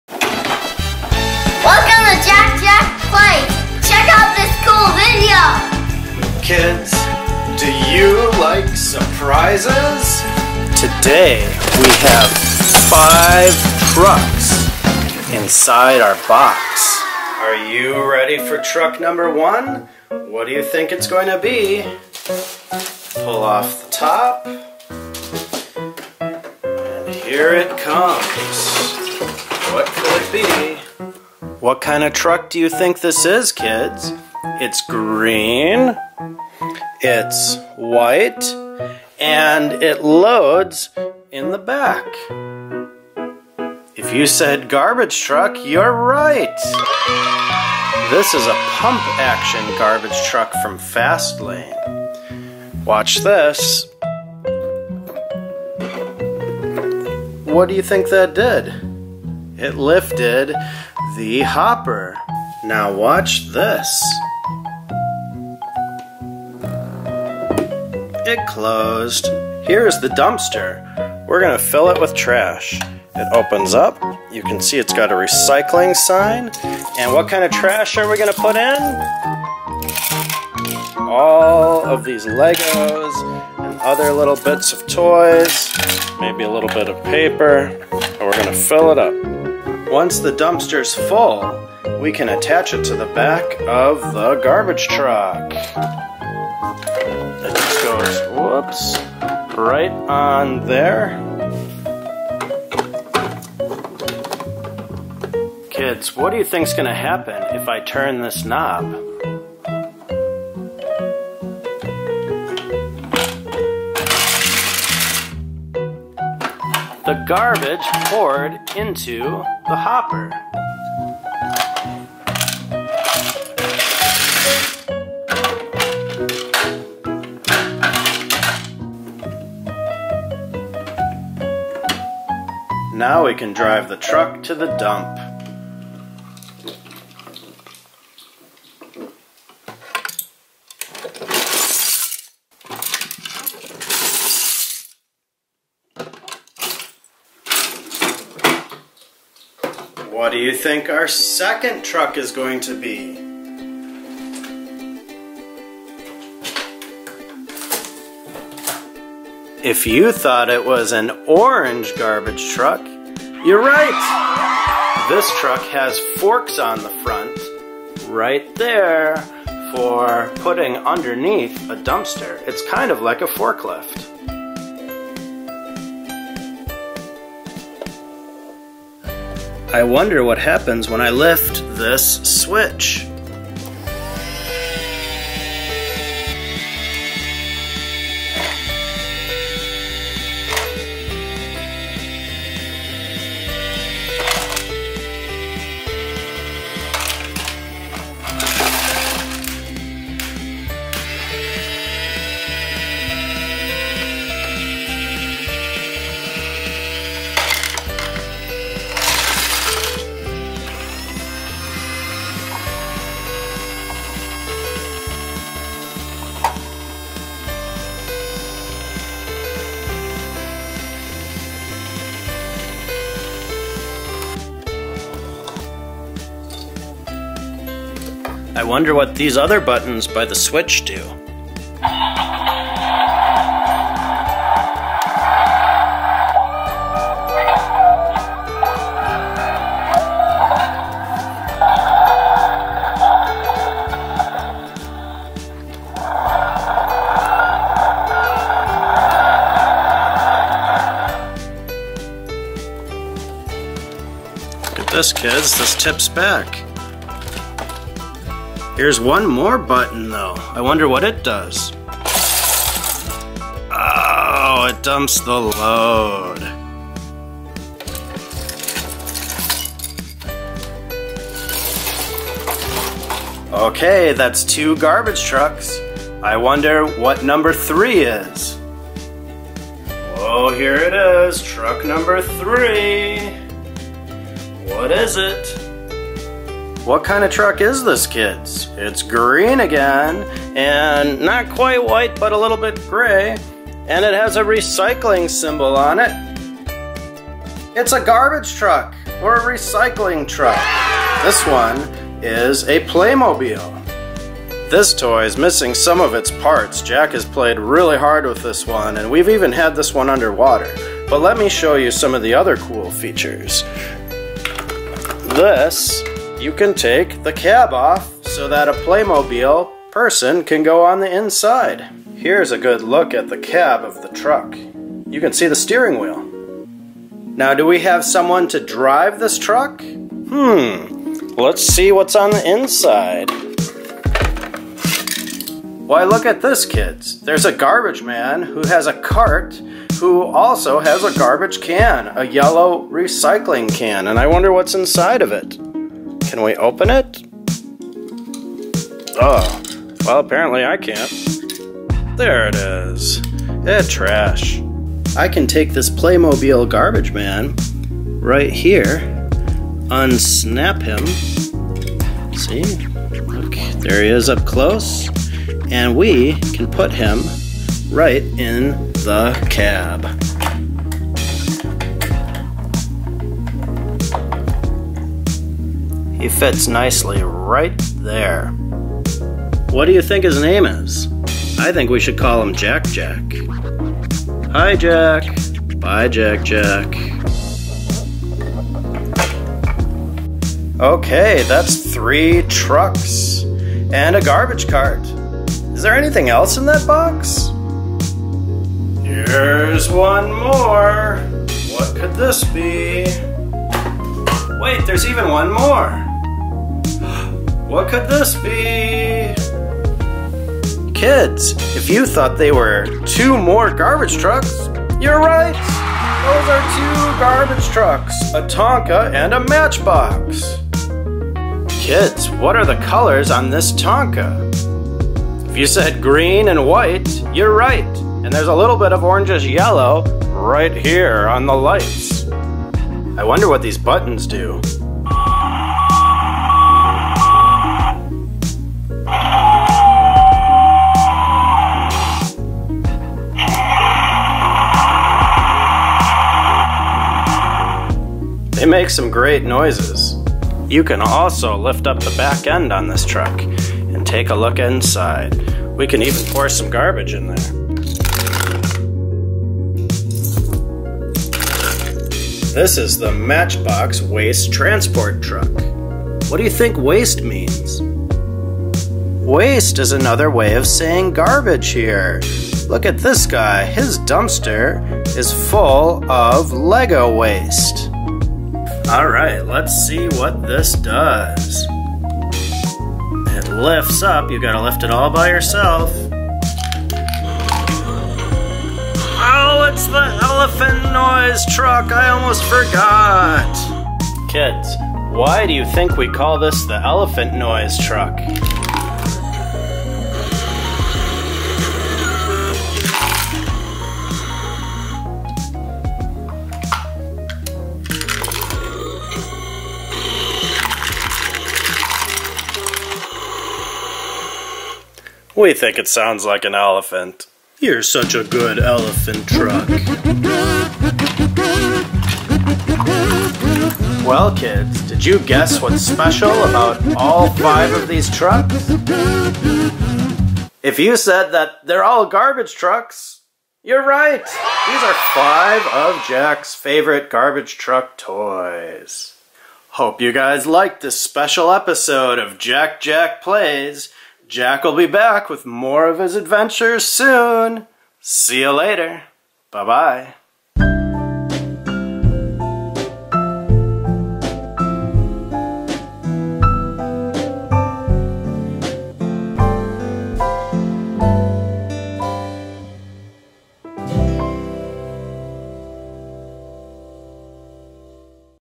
Garbage Trucks for Kids! Surprise Toys Unboxing | JackJackPlays
This fun pretend play episode of JackJackPlays features 5 garbage trucks for kids and an unboxing of new surprise toys! This is our new series "Daddy's Toy Review," where Jack Jack plays with his lego and truck collection! Let's recycle!